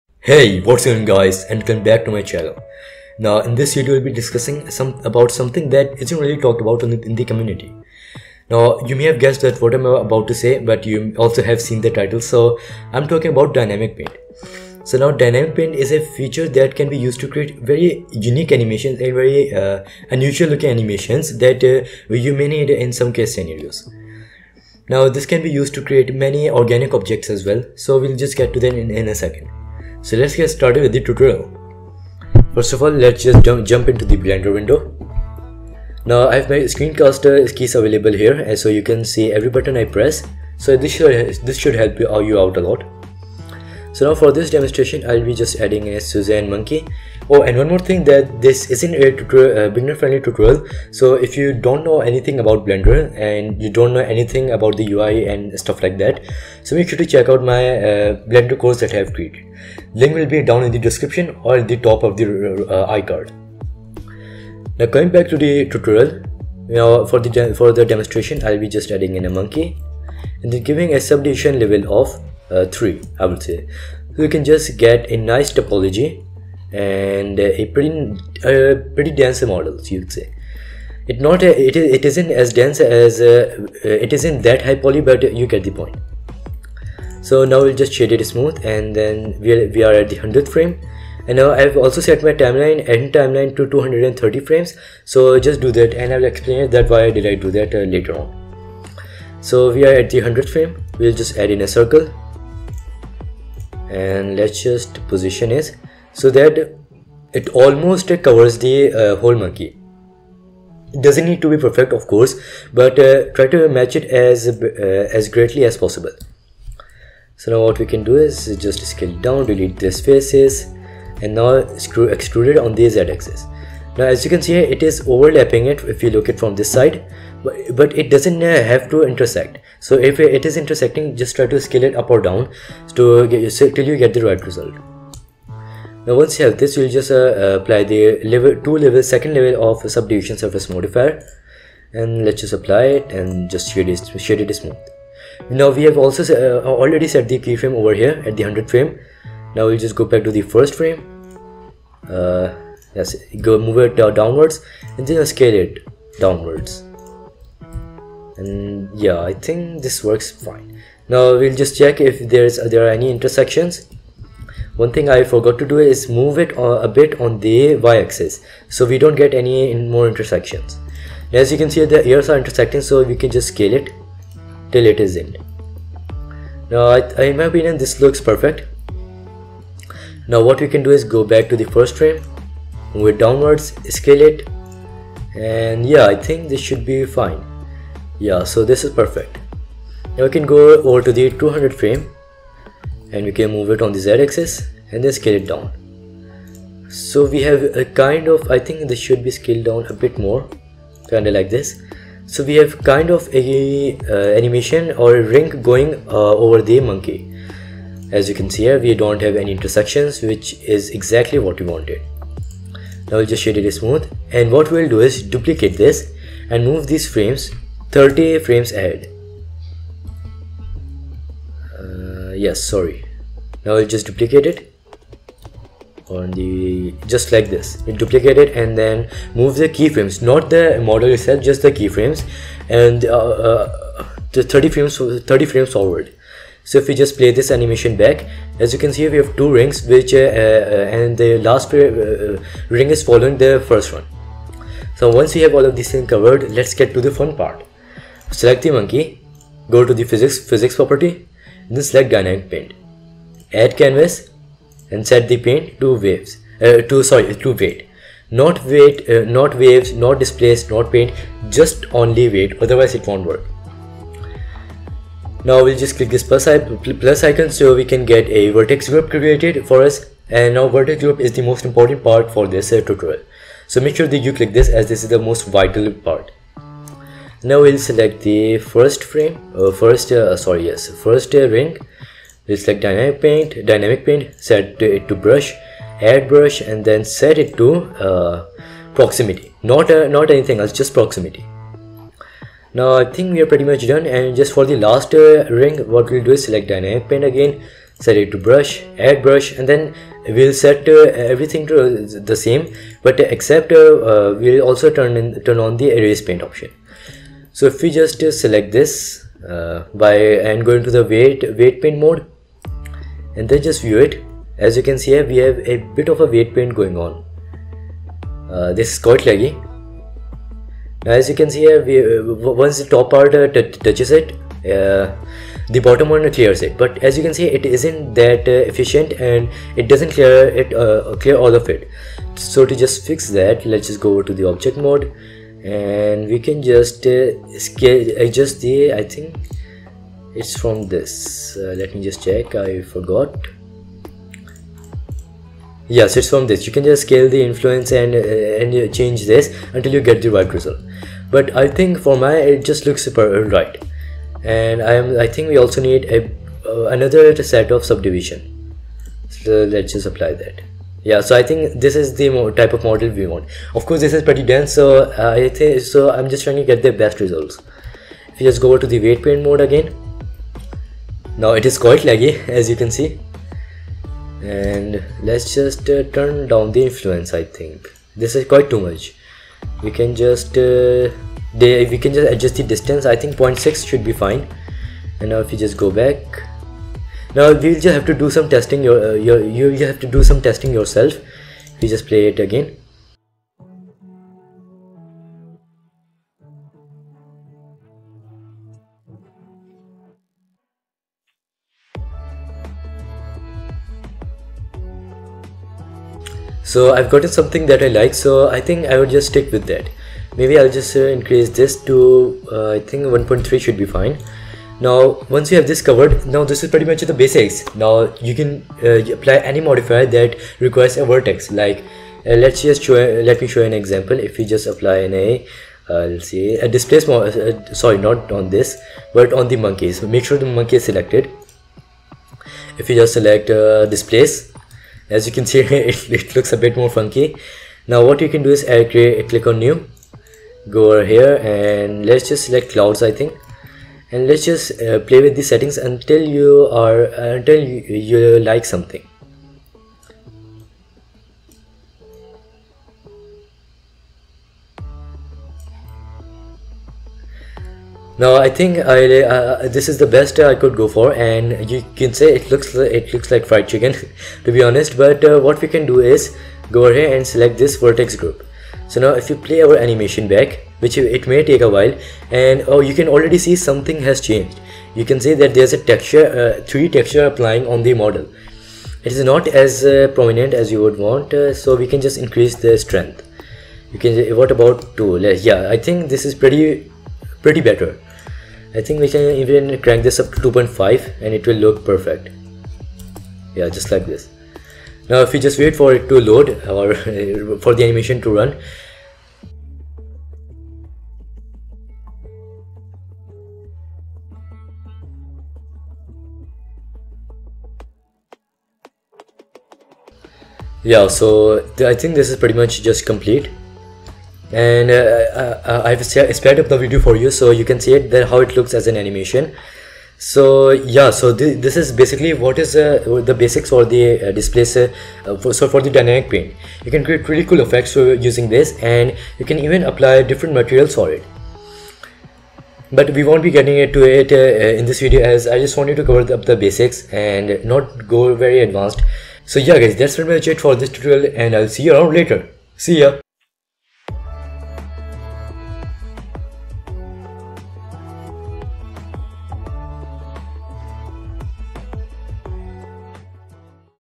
The Most UNDERRATED Feature In Blender!
In this video I show the functionality of dynamic paint, which is one of the most underrated features in blender.